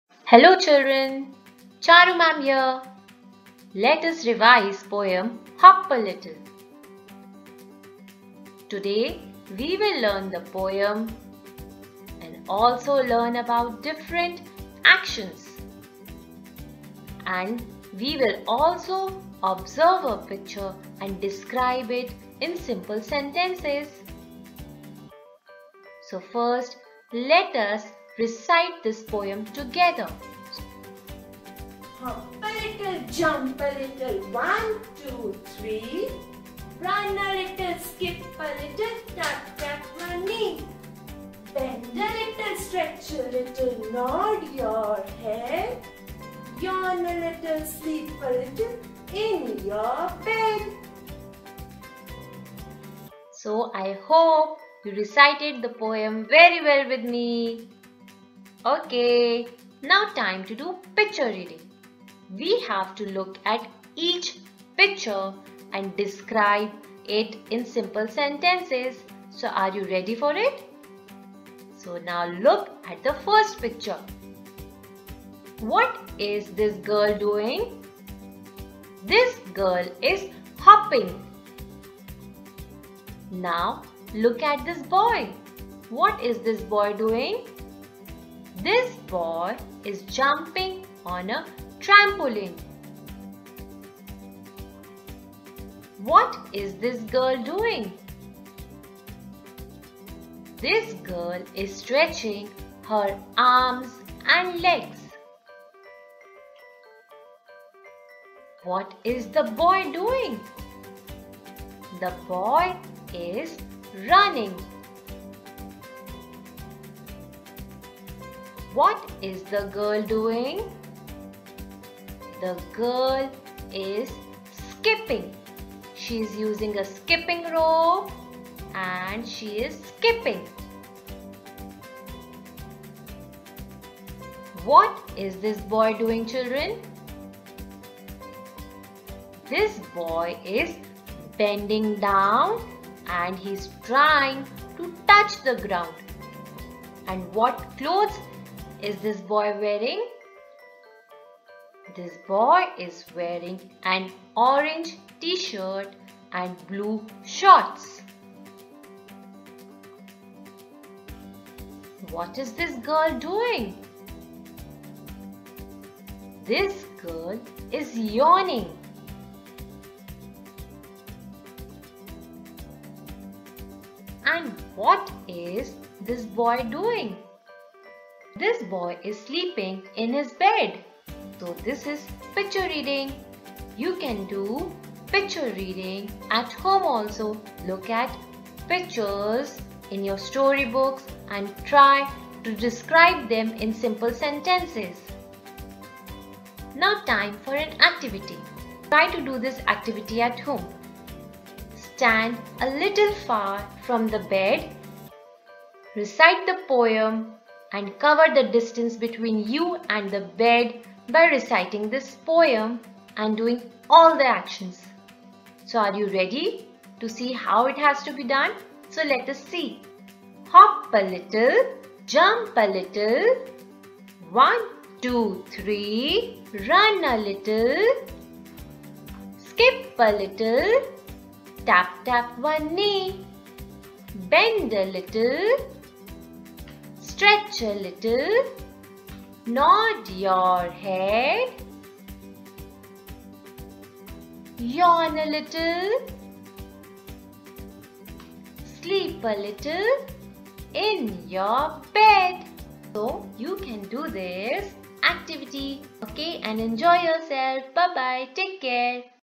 Let's revise the poem ' Hop a Little'
Revising the poem and all the action words